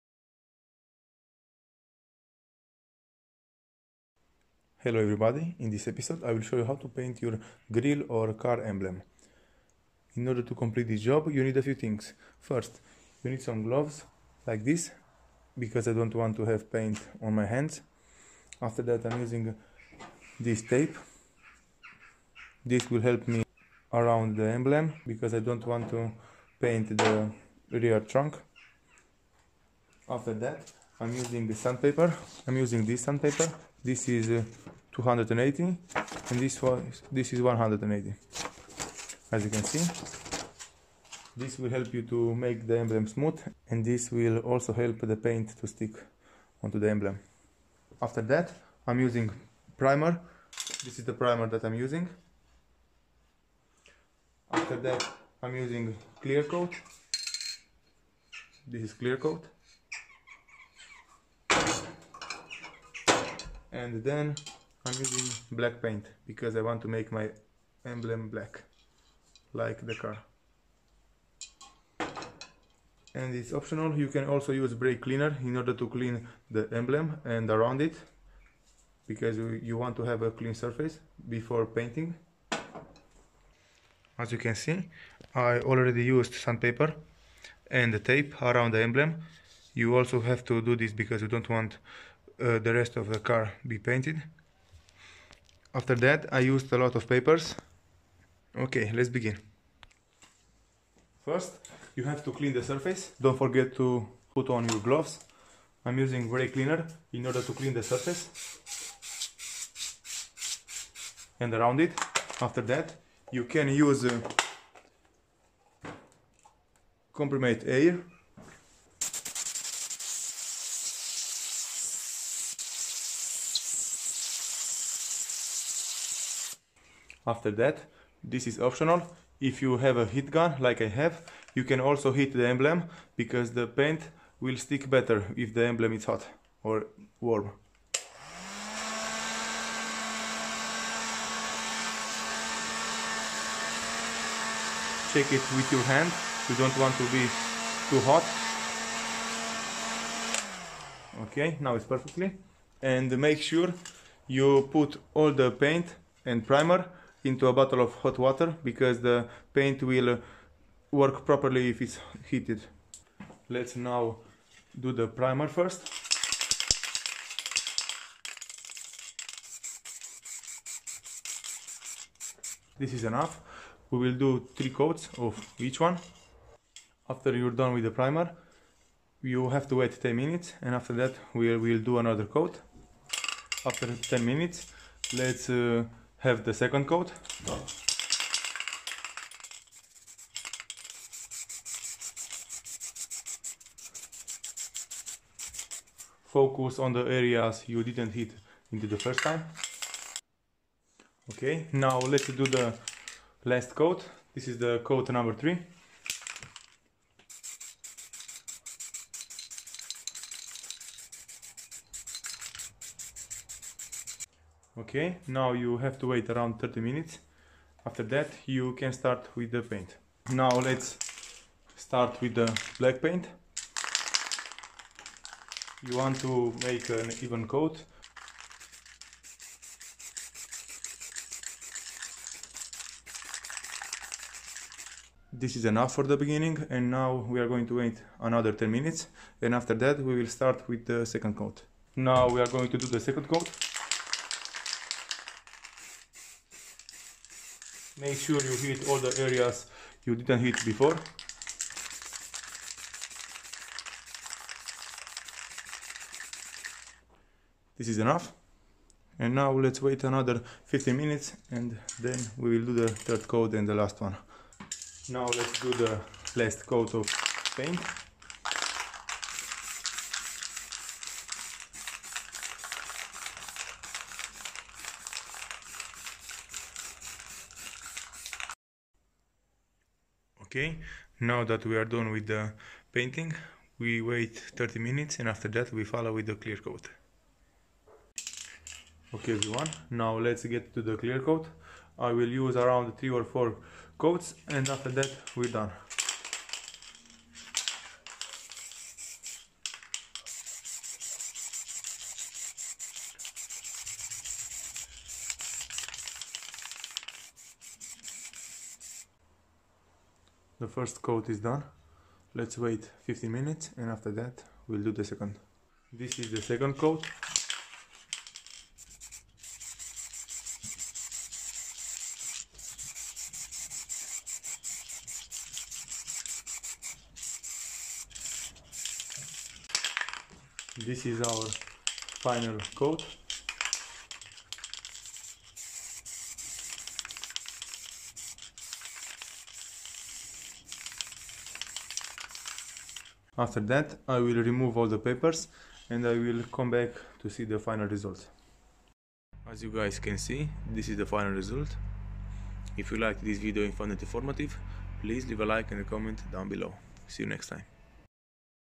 How To Paint Your Car Emblems STEP BY STEP!
Hello everyone, in this episode I will show you how to paint the grille or emblems of your car at home and very simple, follow this steps and you will have the best results🙏